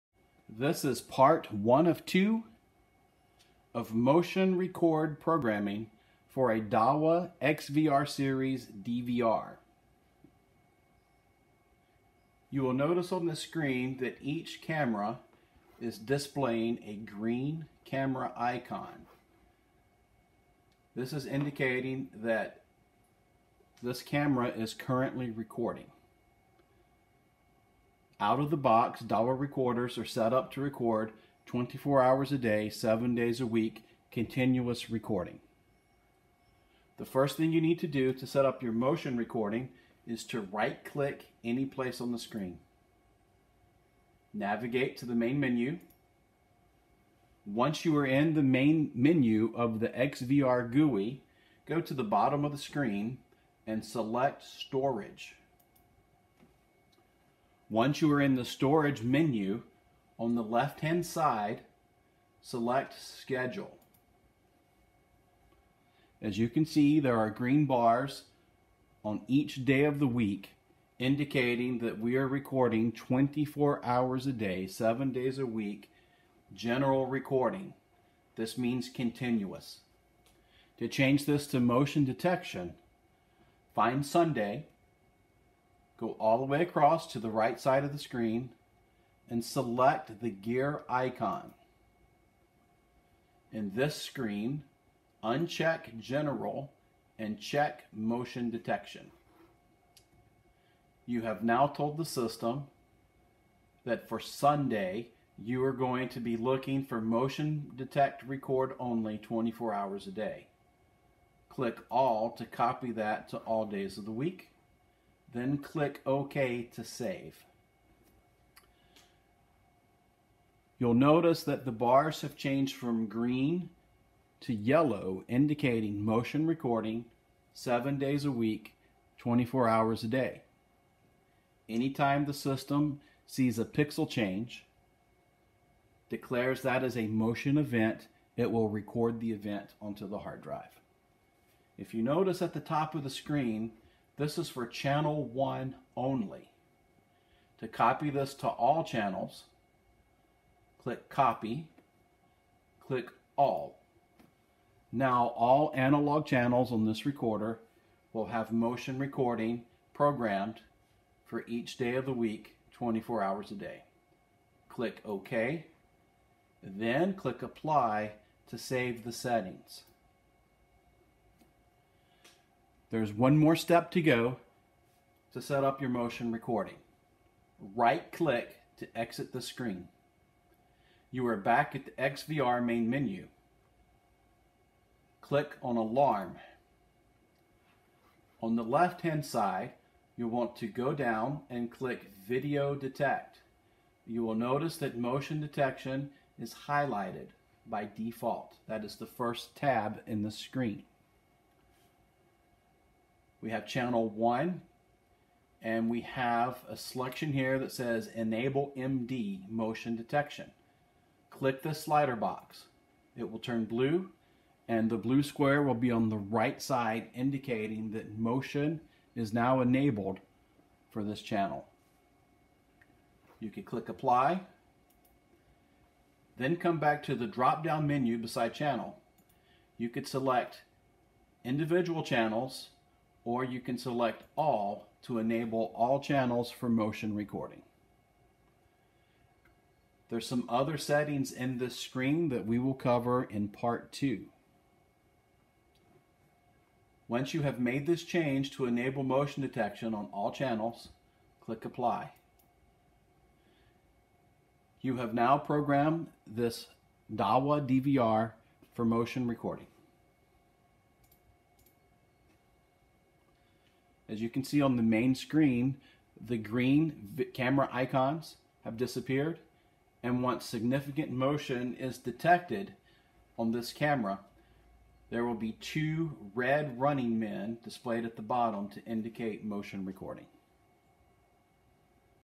Part 1 Dahu XVR Motion Record Programming
Enable motion recording on Dahua XVR series recorders.